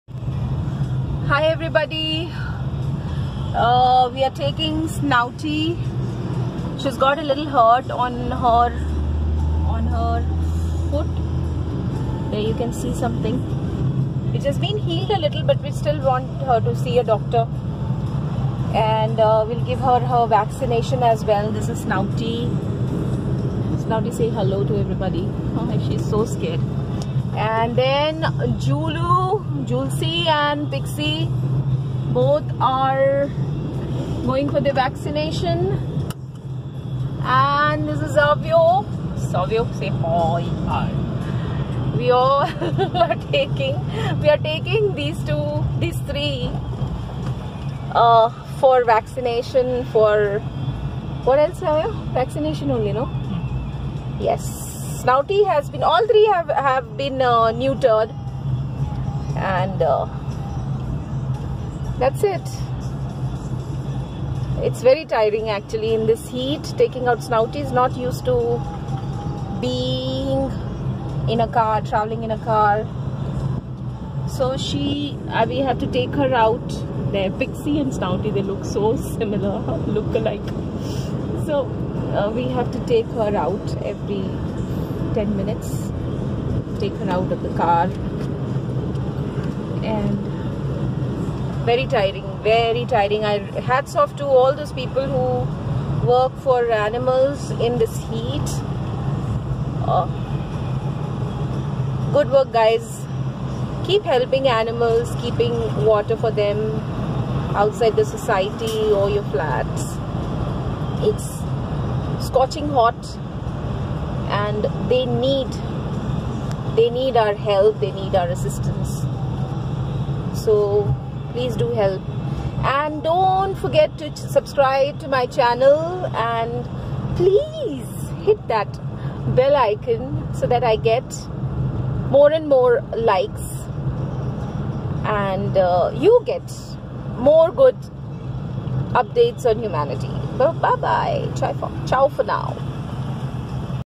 HinjewadiPhase2 indiPups FeedingDogs FeedingAnimals Saint LoveAnimals HinjewadiStrayDogs
Snouty is going for her second Vaccination with my Pixie n Julie... Someone had hit Snouty on her hind foot...it has almost healed but showing it to a doc as well!! PL help animals...🙏🙏❤️